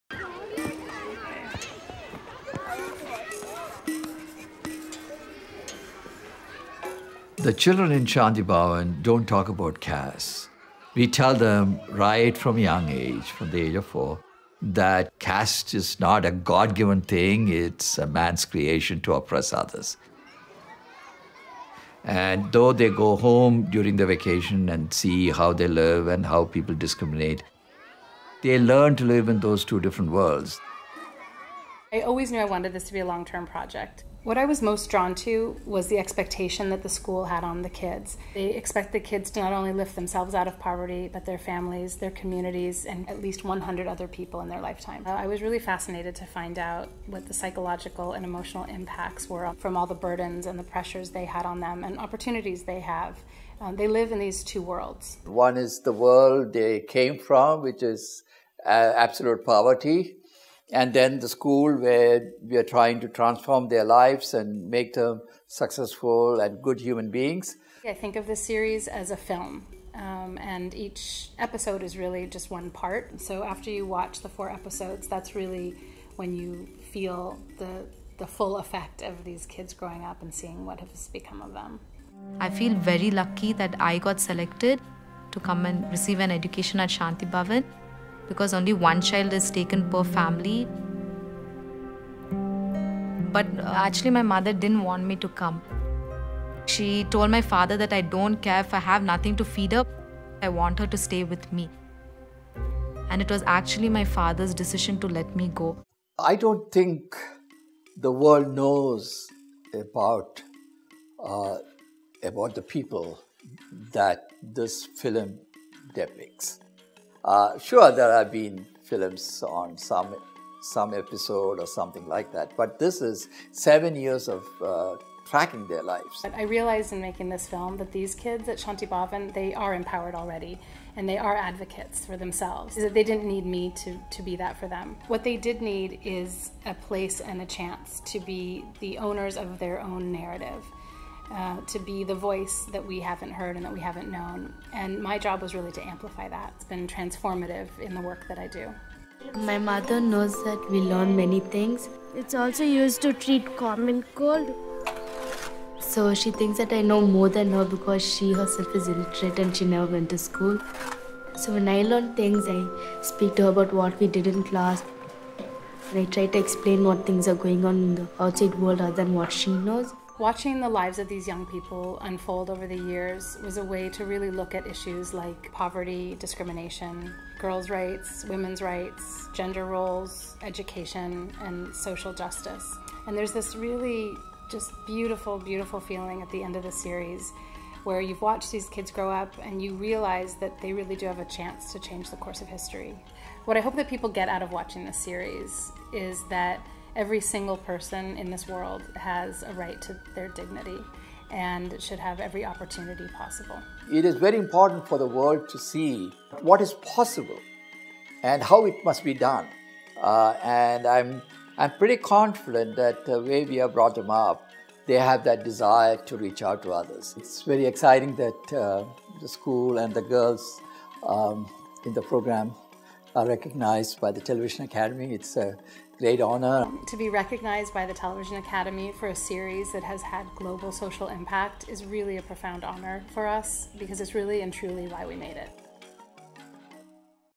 Television Academy Honors Daughters of Destiny
Daughters of Destiny: The Journey of Shanti Bhavan receives a 2018 Television Academy Honors Award.